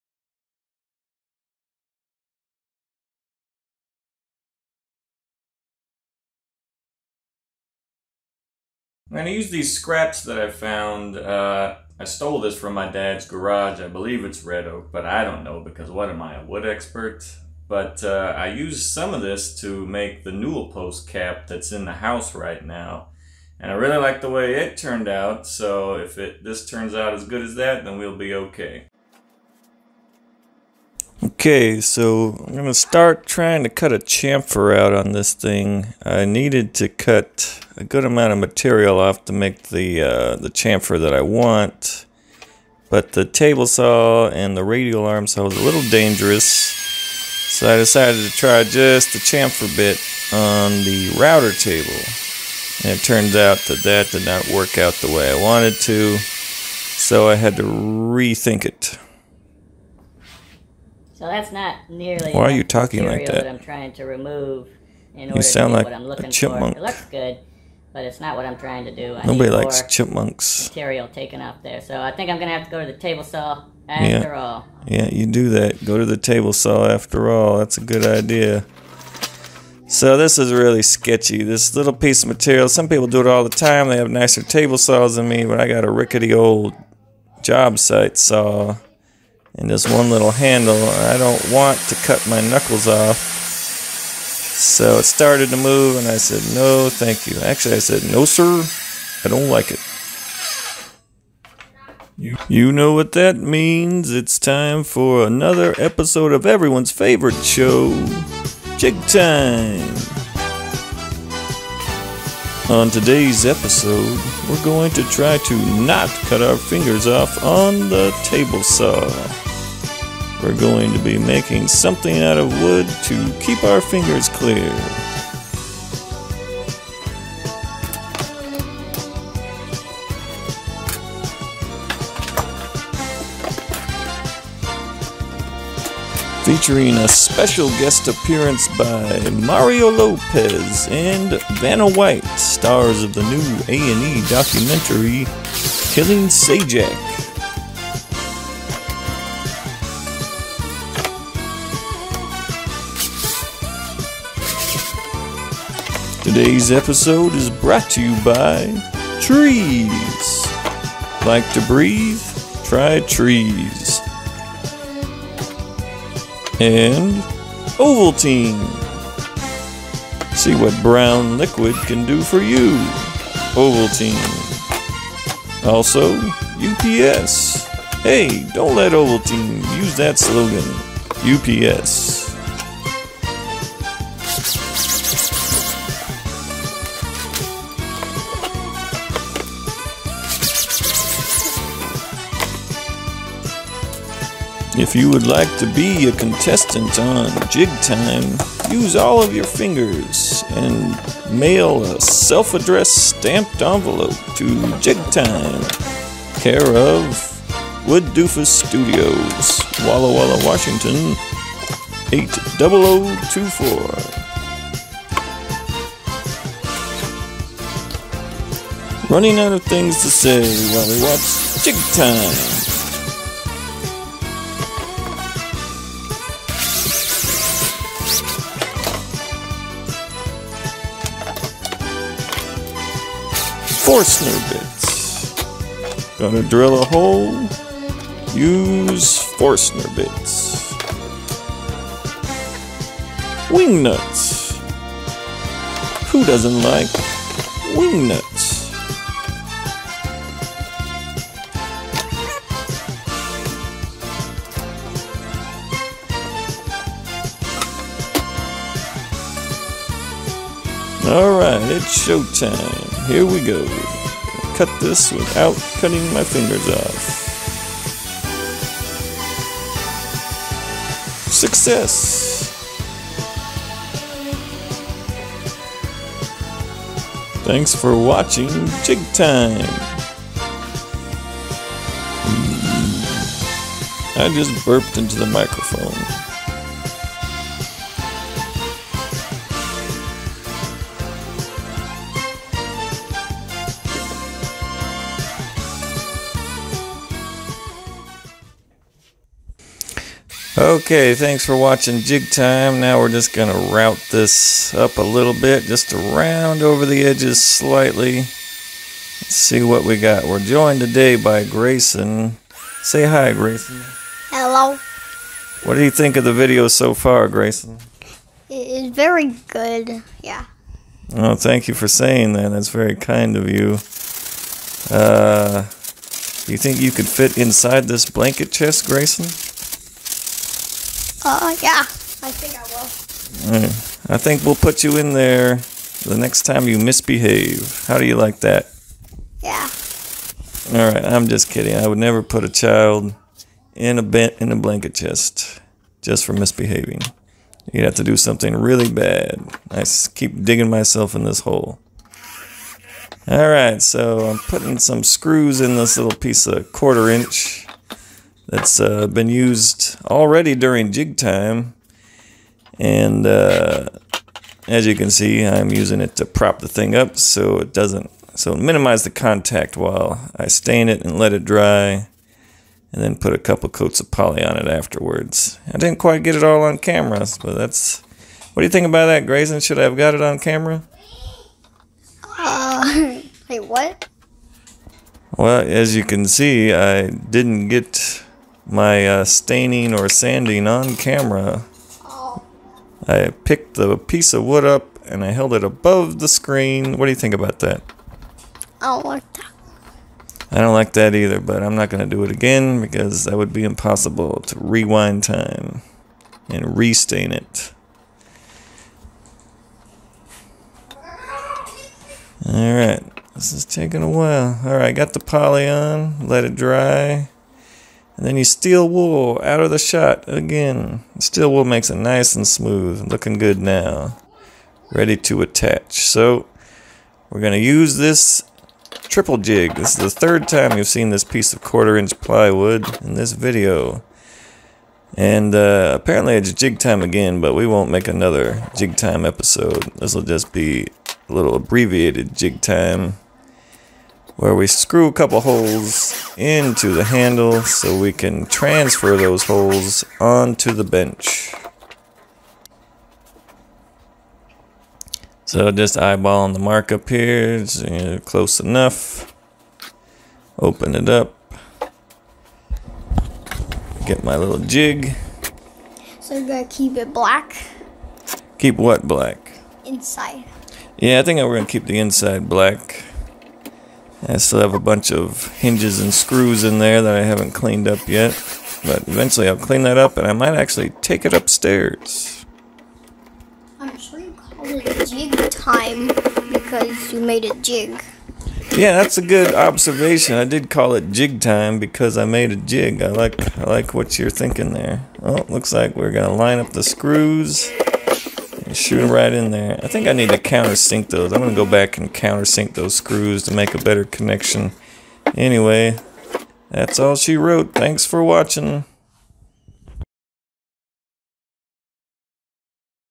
DIY Handle for a Blanket Chest
I finished making my blanket chest in the last video but I still needed a handle. So I decided to save the handle for an additional video. If you want to see how I made the chest, be sure to check out my last video.